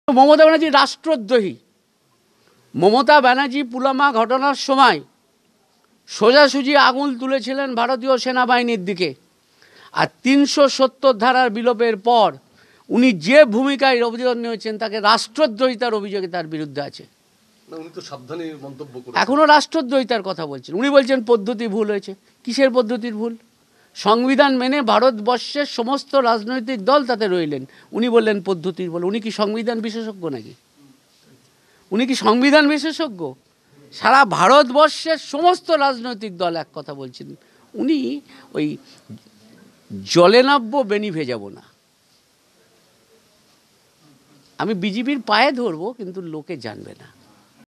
PURULIA MAMATA DESODROHI MUKOL ROY/‘মমতা ব্যানার্জীকে দেশদ্রোহী অখ্যা দেওয়া উচিৎ’ মুকুল রায়।
‘মমতা ব্যানার্জীকে দেশদ্রোহী অখ্যা দেওয়া উচিৎ’ মুকুল রায়।

পুরুলিয়া, ৯ আগস্টঃ মমতা ব্যানার্জীকে দেশদ্রোহী আখ্যা দেওয়া উচিৎ। পুরুলিয়ার একটি বেসরকারি হোটেলে সাংবাদিকদের মুখোমুখি হয়ে মুখ্যমন্ত্রী মমতা ব্যানার্জীকে এভাষাতেই কটাক্ষ করলেন বিজেপি নেতা মুকুল রায়। এদিন ৩৭০ ধারা বিলোপ প্রসঙ্গ তুলে মুকুল বাবু বলেন, পুলওয়ামা ঘটনার পর দিদি সরাসরি কেন্দ্রীয় বাহিনীর দিকেই আঙ্গুল তুলেছিলেন। আর ৩৭০ ধারা বিলুপ্তির পরও পদ্ধতিগত ভুলের অভিযোগ করছেন তিনি।

এদিনের বৈঠকে ‘দিদিকে বলো’ কর্মসূচী নিয়েও শাসকদলকে কটাক্ষ করতে ছাড়েননি মুকুল রায়। তাঁর দাবি পিসি-ভাইপোর কোম্পানি থেকে মানুষের নজর ঘোরাতে এখন পিসি থেকে, দিদি হতে চাইছেন মমতা ব্যানার্জী। এমনকি মেন্টর হিসেবে প্রশান্ত কিশোরের প্রশিক্ষণের পরও তিনি তো বদলাননি, বরং আরও খারাপ হয়েছেন বলে দাবী করেছেন মুকুল রায়।